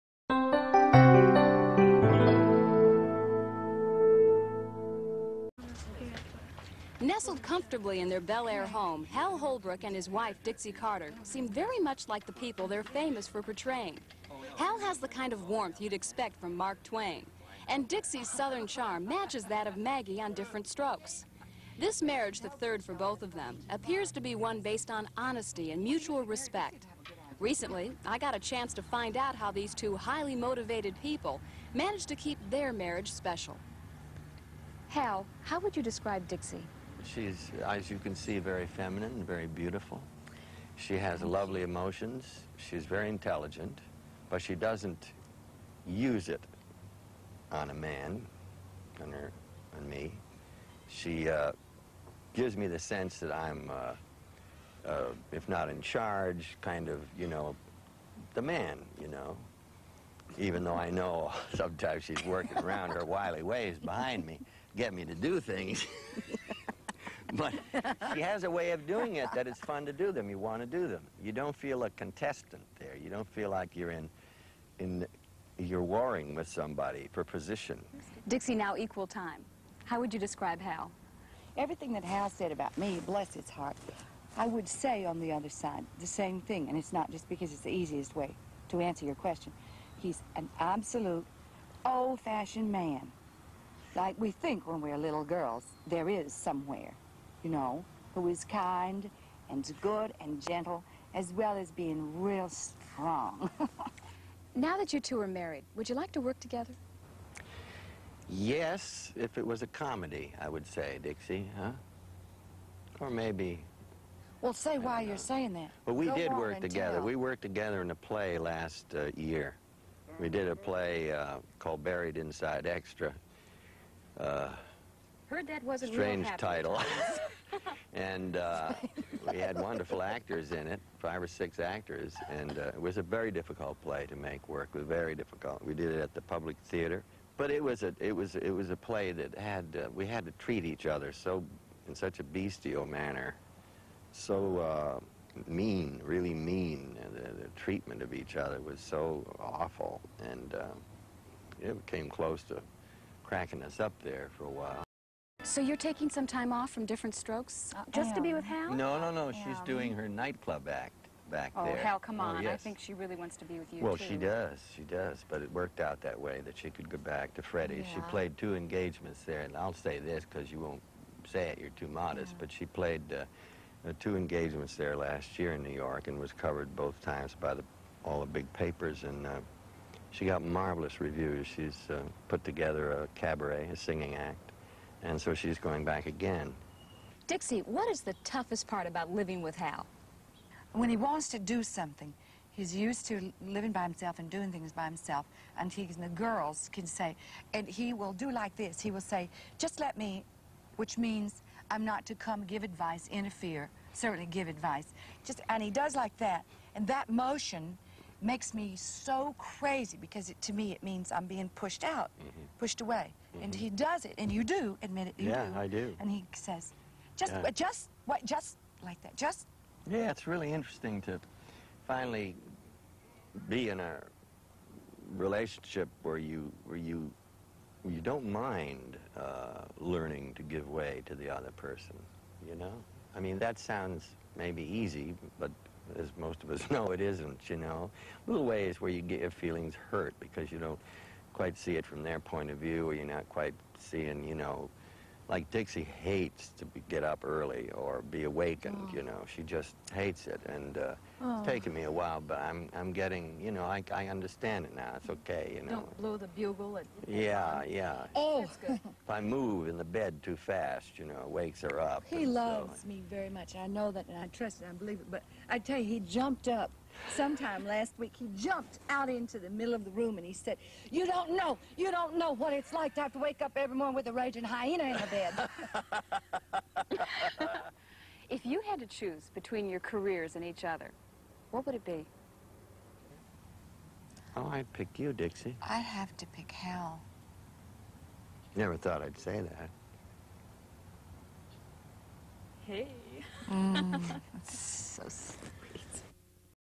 Hal Holbrook interview with wife Dixie Carter at home 1984
Hal Holbrook interview with wife Dixie Carter at home 1984. Newlyweds Hal Holbrook and Dixie Carter interviewed at home in Bel Air, Calif. Actor Holbrook was best known for his portrayal of Mark Twain and actress Carter became well-known for her leading roles in TV series "Designing Women" and "Family Law." Hal Holbrook died on January 23, 2021 at age 95, preceded in death by Carter who passed away in 2010.